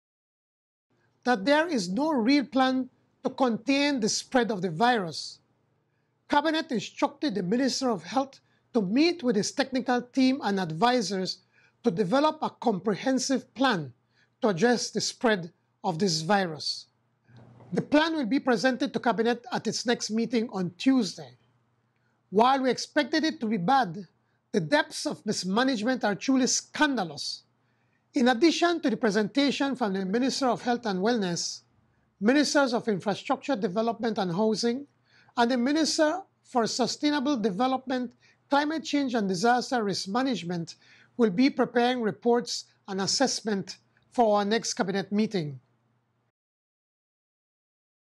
PM Briceño: Former Administration Had No Real Plan to Contain COVID-19 Spread
That briefing was with senior officials, including Commissioner of Police Chester Williams, Central Bank Governor Joy Grant and Financial Secretary Joseph Waight. Another big challenge facing the government is the COVID-19 pandemic. PM Briceño says after meeting with Director of Health Services, Doctor Marvin Manzanero, it was realised that the previous administration had no real plan to contain the virus in Belize.  Here is the PM on that situation.